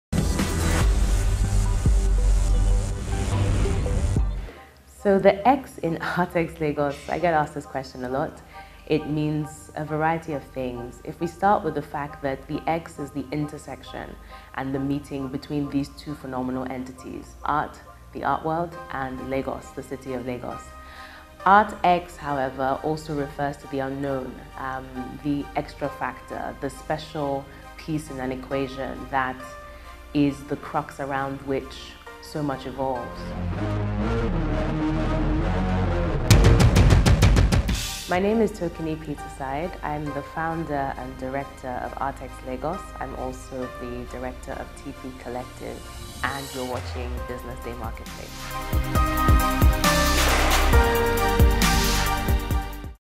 BusinessDay speaks with Tokini Peterside on BusinessDay Marketplace
Listen to Tokini Peterside talk about Art in Today's Nigeria.